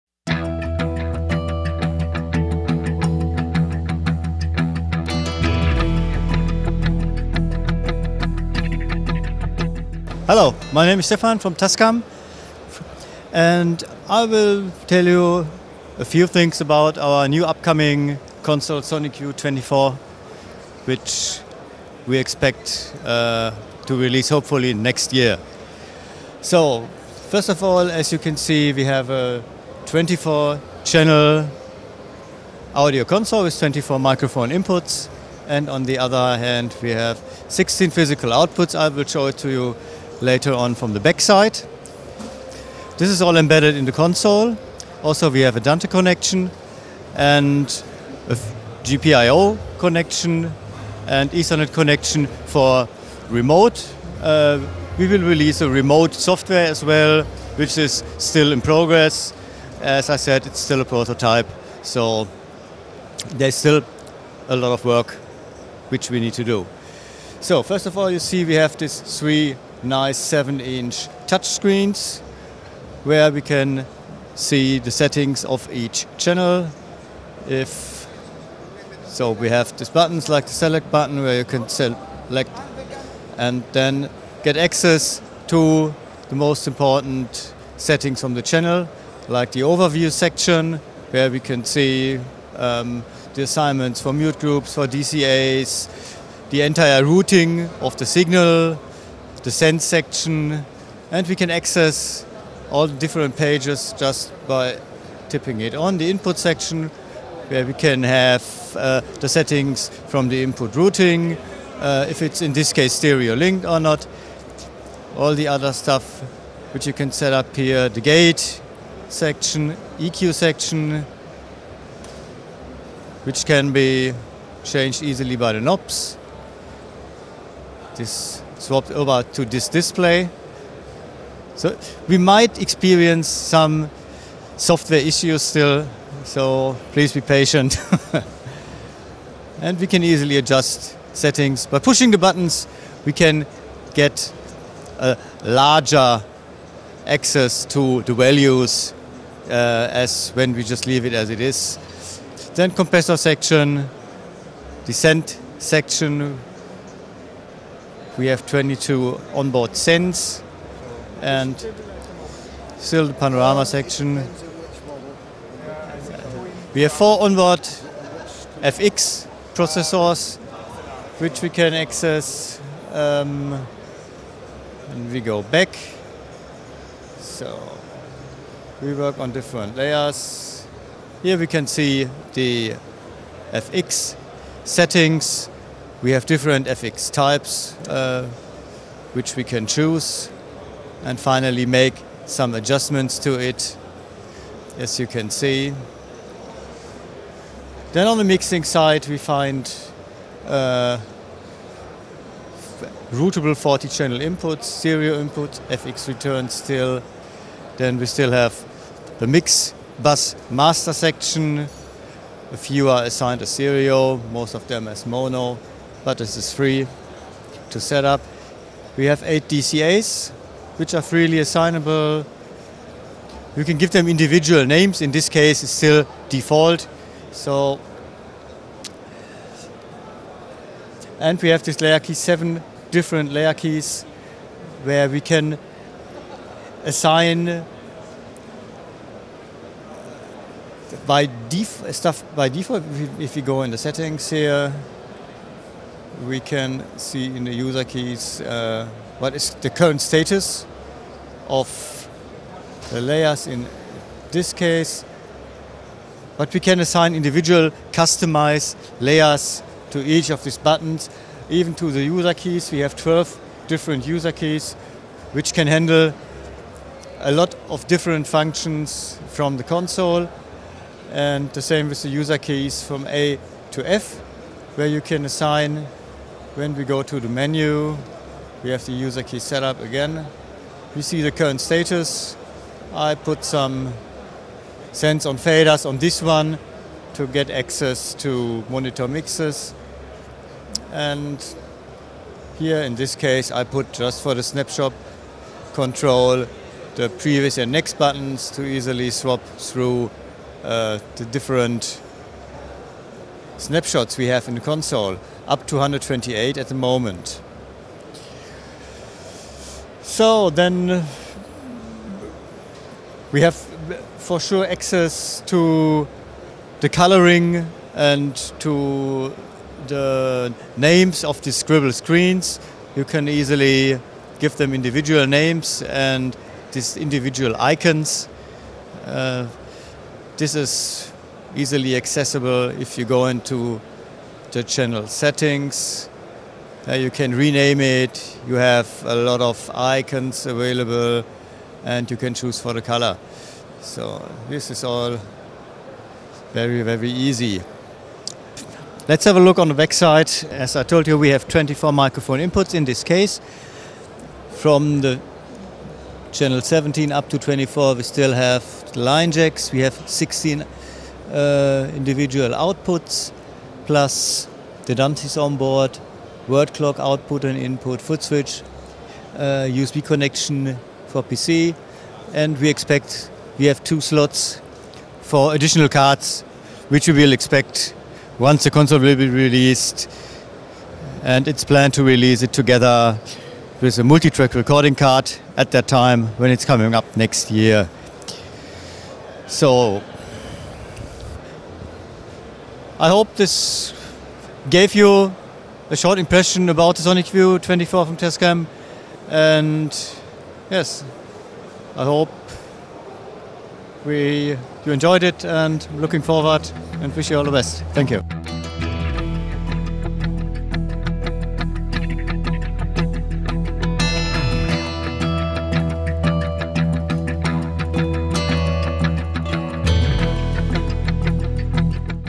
Tascam Sonicview 24 - nowy mikser cyfrowy [ISE 2022]
Tascam, uznany producent urządzeń studyjnych, dołączy do grona producentów cyfrowych konsolet audio wraz z nowo zaprezentowanym na targach ISE 2022 modelem miksera cyfrowego Sonicview 24. Celowo piszemy prototyp, gdyż konsoleta przechodzi liczne testy, usprawnienia i premiera oficjalna zaplanowana jest na 2023 rok. Nic nie szkodzi jednak na przeszkodzie aby oznajmić światu nowy rozdział w historii firmy. Zapraszamy do relacji video prosto z ISE 2022.

Nowa konsoleta cyfrowa: Tascam Sonicview 24
Sonicview 24 ma 24 wejścia i 16 wyjść analogowych oraz interfejs Dante. Mikser może przyjąć i obsłużyć 40 sygnałów wyjściowych, wyprowadzić zaś można 22 sygnały z szyn wyjściowych. Na pokładzie mamy 4 procesory FX, dysponować będziemy też 8 grupami mutowania i 8 grupami DCA. Dodatkowe 2 sloty na karty rozszerzeń pozwolą zwiększyć wybór rodzajów sygnałów podawany do i z miksera.

00:00 Tascam Sonicview 24 (intro)
00:10 Stefan Ringer prezentuje nowy mikser cyfrowy
00:25 Gdzie kupić Tascam Sonicview 24
01:16 ekran dotykowy i funkcje
03:20 zmotoryzowane suwaki i przyciski funkcyjne
05:48 wejścia / wyjścia (panel tylny) - Dante